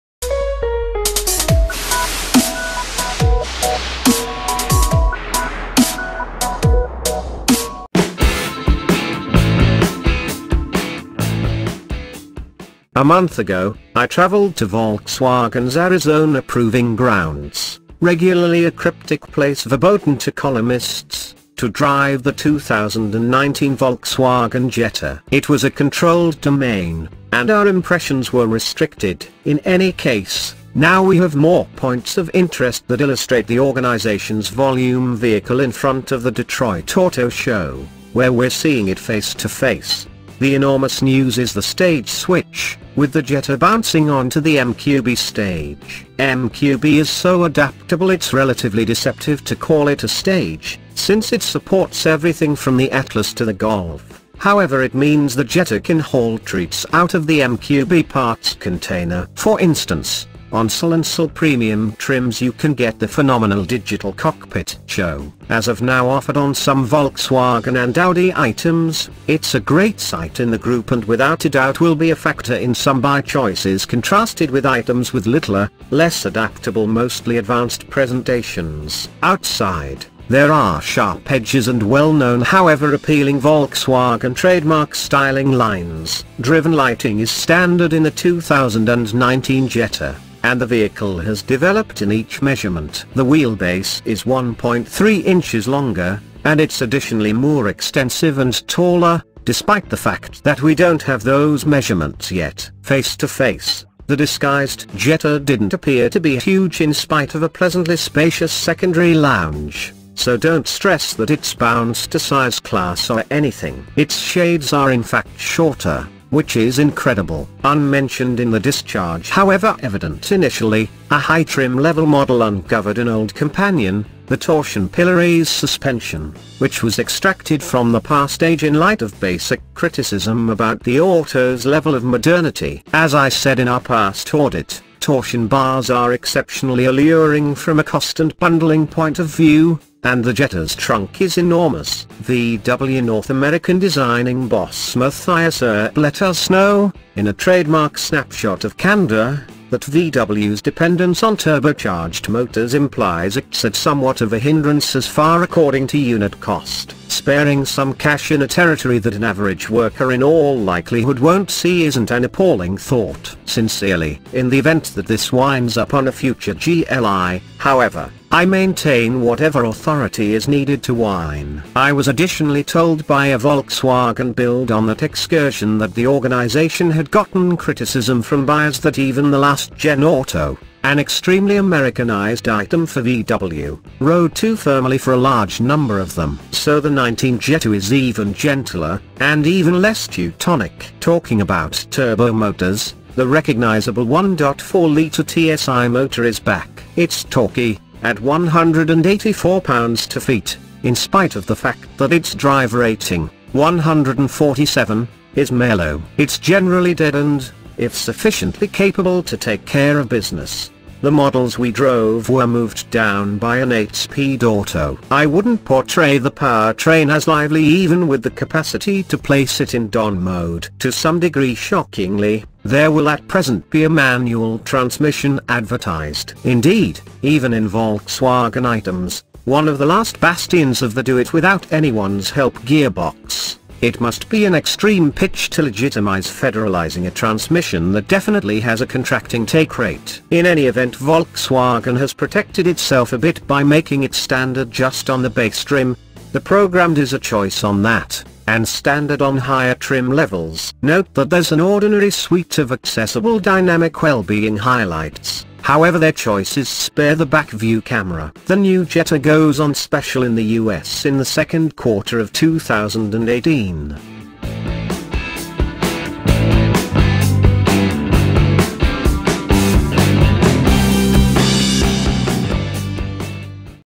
2019 VOLKSWAGEN JETTA PROTOTYPE REVEALED | MORE TECH , LESS TEUTONIC
Last month, I flew to Volkswagen's Arizona Proving Grounds — normally a secretive place verboten to journalists — to drive the 2019 Volkswagen Jetta gli and redesign. It was a controlled environment, and our impressions were limited. But now we have more details that paint a clearer picture of the company's volume sedan ahead of the Detroit Auto Show, where we're seeing it in person

++ Likes me on Facebook :
++ Follow me on G+ :

Backsound Free Royalty license by The_Contractor_General